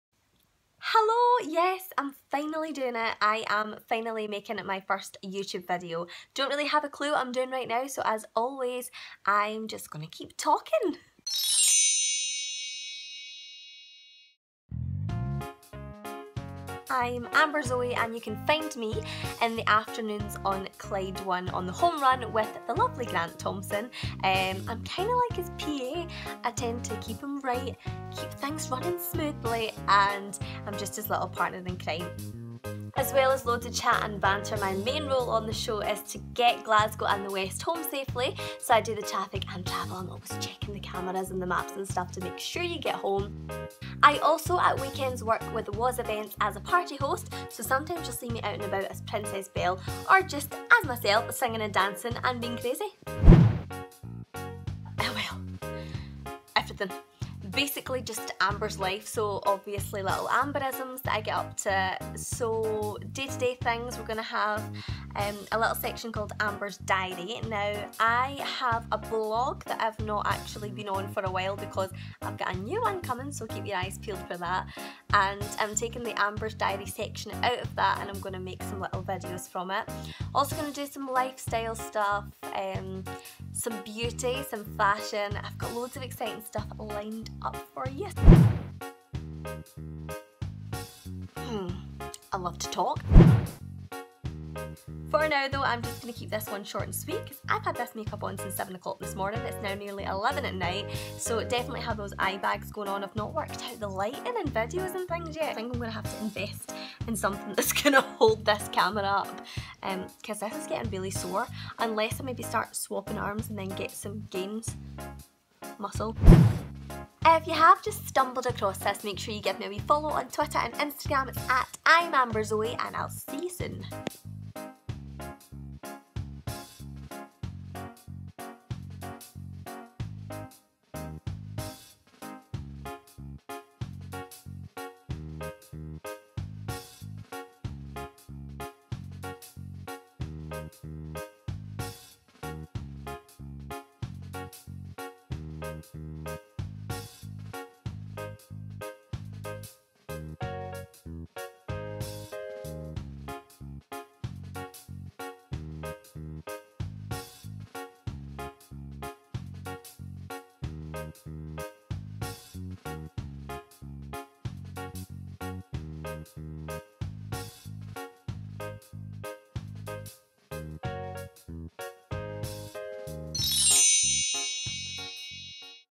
Hello Youtube!
It's happening! I've made my first Youtube video. I'm just a newbie, but I'm so excited to be here!

Here is a little introduction of who I am, and some clips of my 'amberisms'! I can't wait to get started.